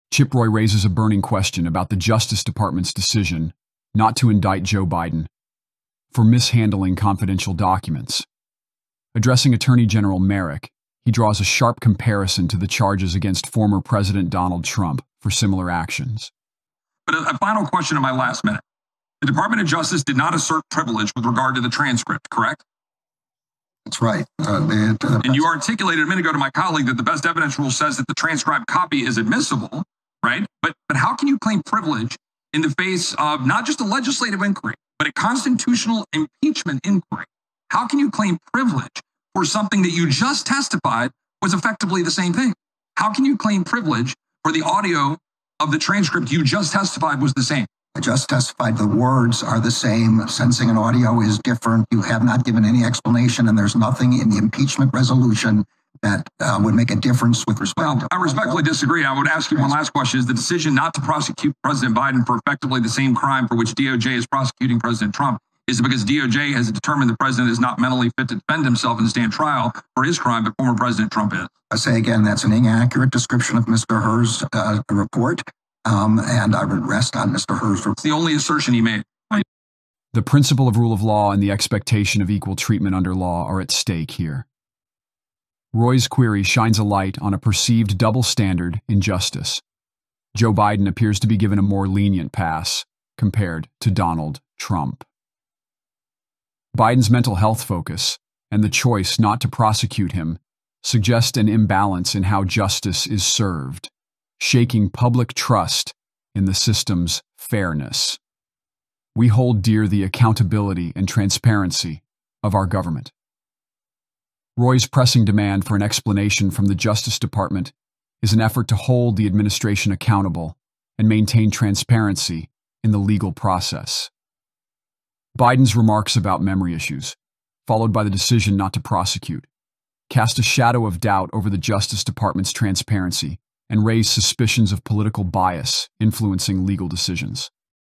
Chip Roy, Questions DOJ's Decision Not to Prosecute Biden
Chip Roy Grills DOJ: Why No Biden Prosecution? In a heated exchange, Chip Roy questions Attorney General Merrick Garland on why the DOJ has not prosecuted President Biden for allegedly the same crime for which former President Trump is being prosecuted. The discussion delves into the DOJ's rationale, questioning whether Biden's mental fitness is being used as a defense. This video provides an in-depth analysis of the ongoing debate over judicial fairness and political accountability. Watch as Chip Roy demands answers and challenges the DOJ's handling of these high-profile cases.